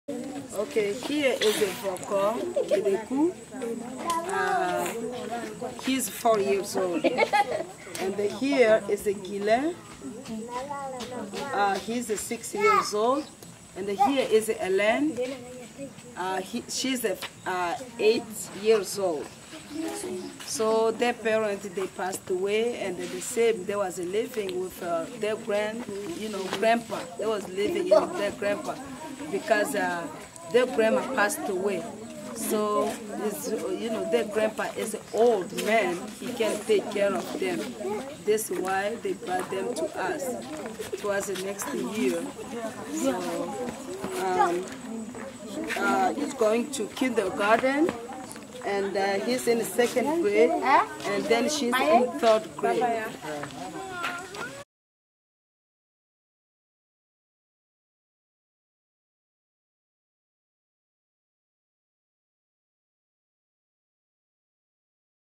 Foco, Helen, Gylin
Meet Foco, Gylin & Helen whom are supported at the Children's Center in Gemena, DR Congo. Interviewed in May 2014.